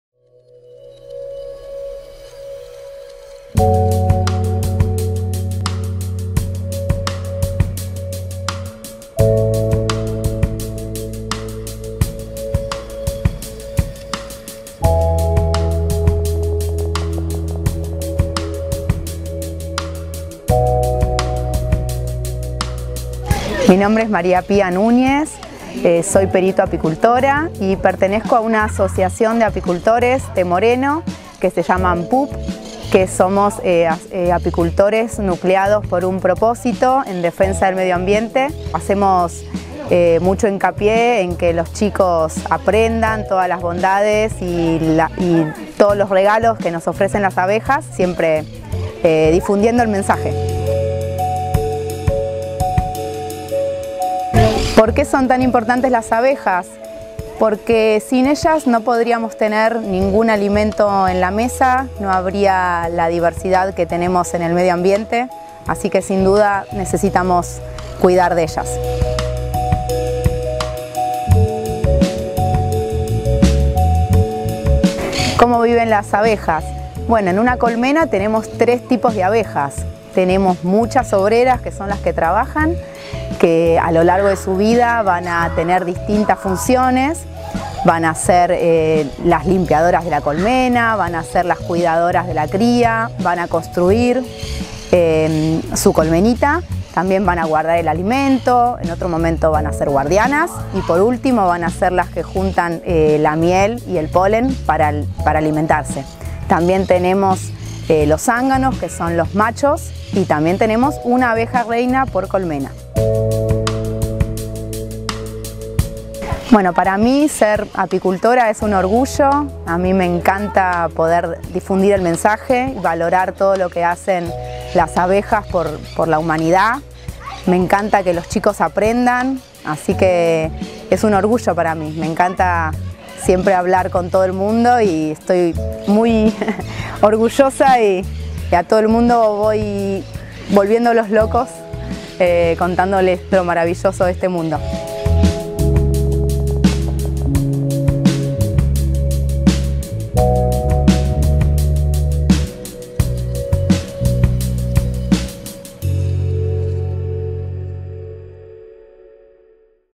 2022 07 16 Taller de Ciencia: Las ABEJAS y su odisea por la miel.
Se realizó el pasado fin de semana un nuevo Taller de Ciencia para la Familia. En esta oportunidad, nos visitó la Perita Apicultora María Pía Núñez, quien nos transmitió su pasión por el fascinante mundo de las abejas.